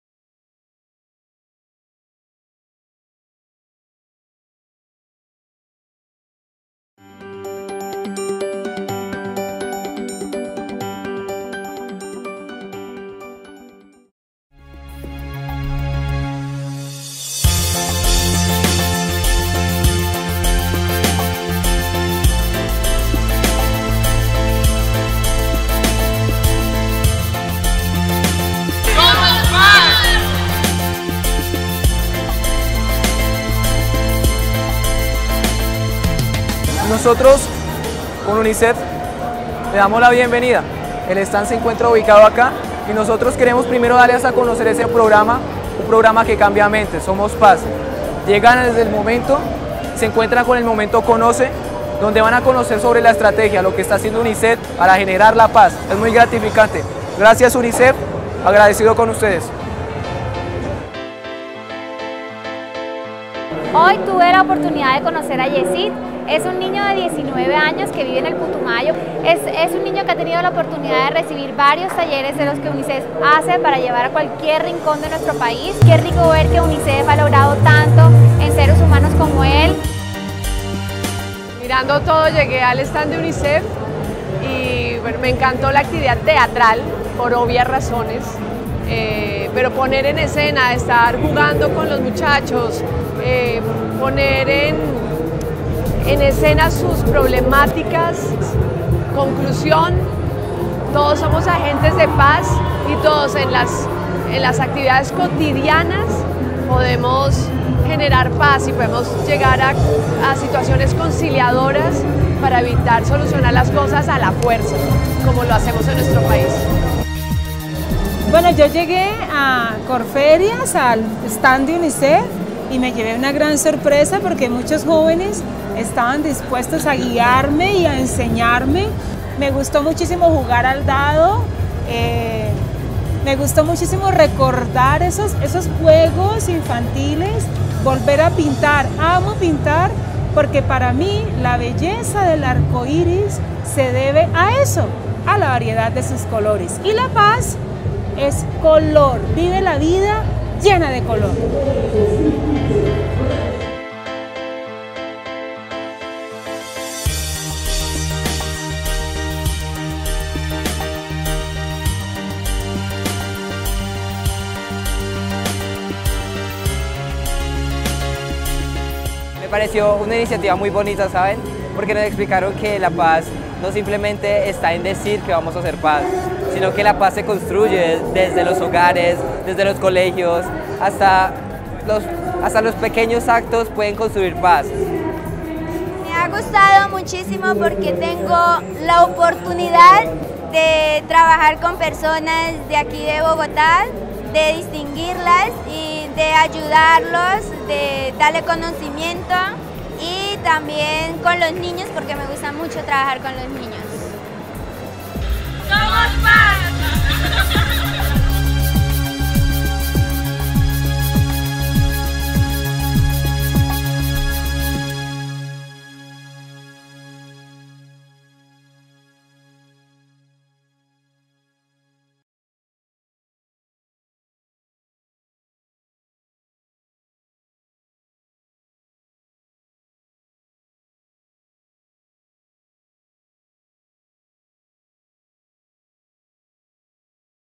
Xpo JóvenEs 205 - Somos paz
En Xpo JóvenEs 2015, UNICEF presentó SOMOS PAZ. Estrategia para incentivar acciones y escenarios de paz con niñas, niños y adolescentes.